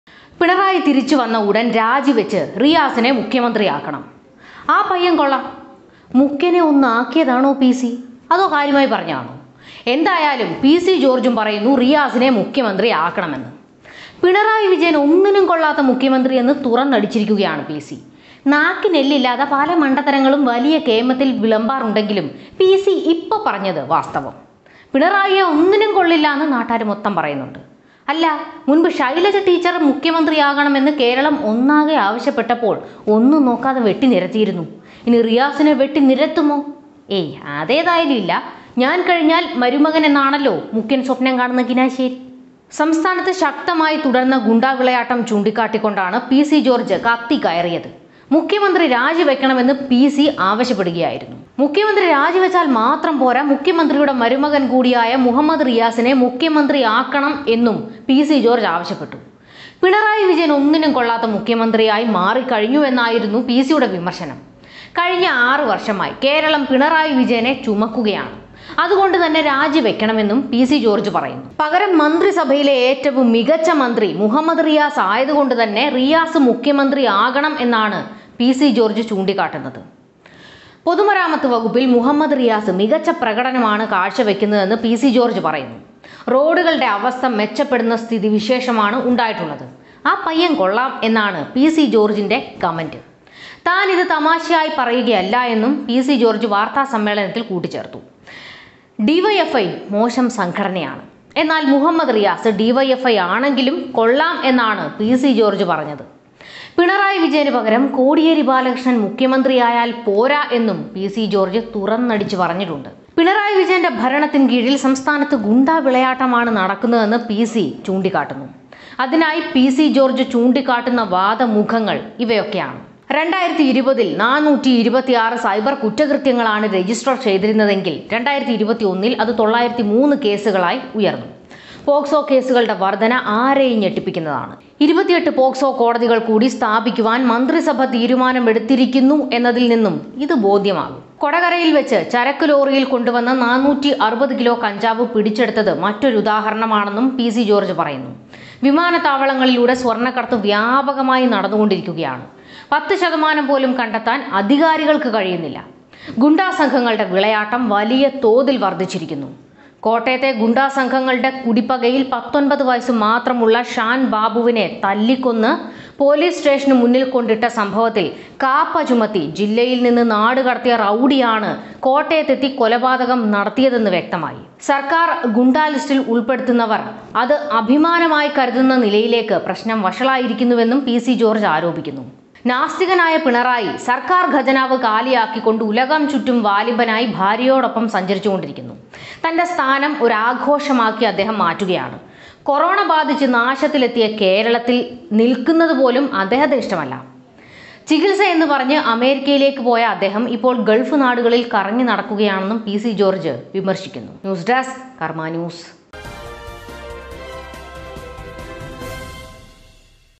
റിയാസ് മുഖ്യമന്ത്രിയാകട്ടെ,ആ പയ്യന്‍ കൊള്ളാം;PC പറഞ്ഞതാണെ
പിണറായി ഒന്നിനും കൊള്ളാത്തവന്‍,റിയാസിന് മുഖ്യമന്ത്രി സ്ഥാനം കൊടുക്ക്,പിണറായിയെ തേച്ചൊട്ടിച്ച് പിസി